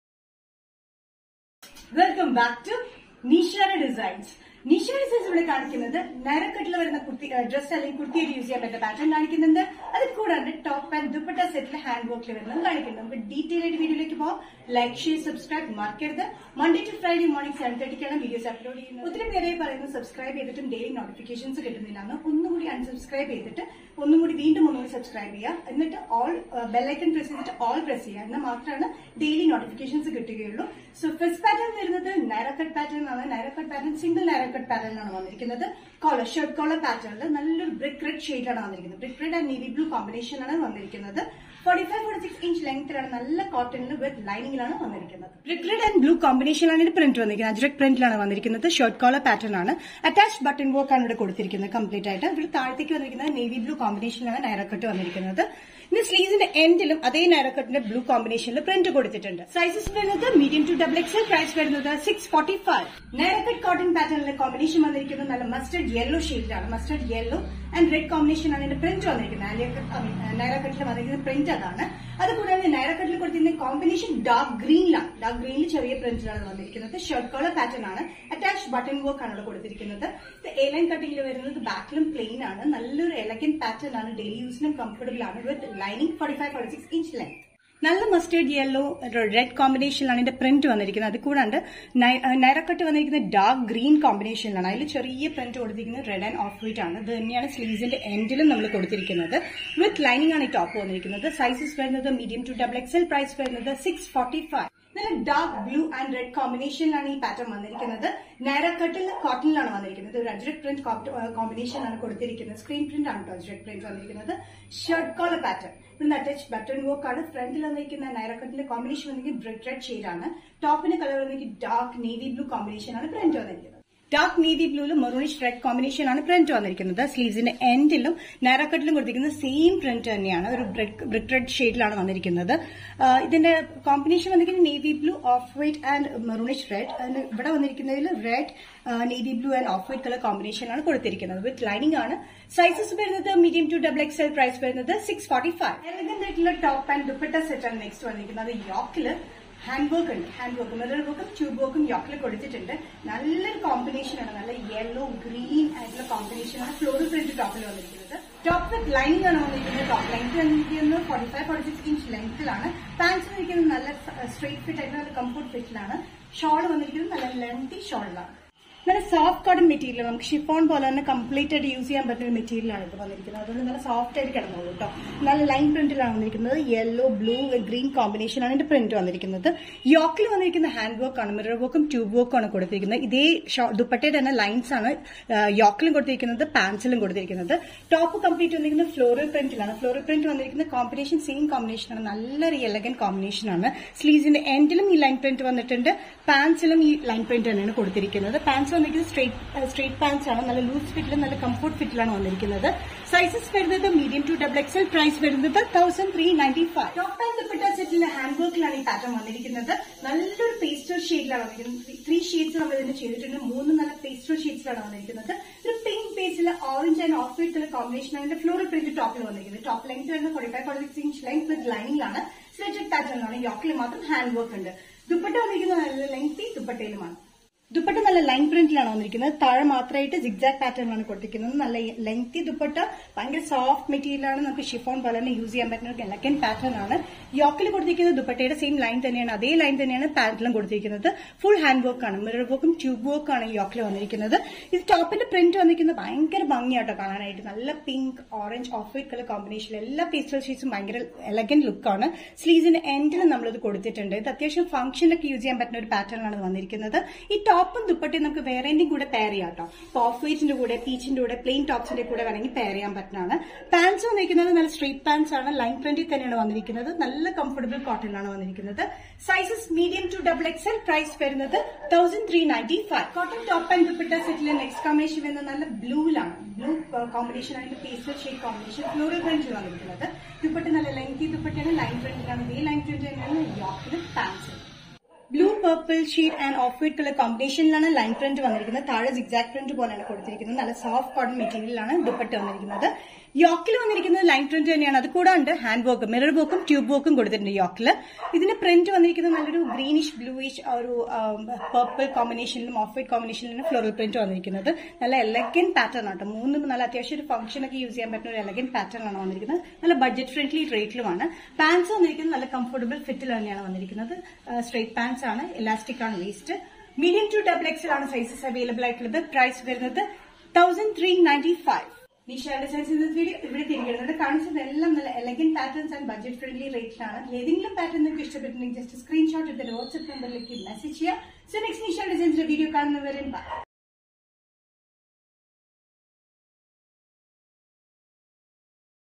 Best Buy ONAM COLLECTIONS | Neeshara Designs
Please watch the video in high picture quality for the clarity of the kurti design.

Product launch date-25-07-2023
Neeshara Designs

Available size: M, L , XL, XXL

We accept Gpay and bank transfer

PLEASE NOTE, ONLY LIMITED STOCKS ARE AVAILABLE!!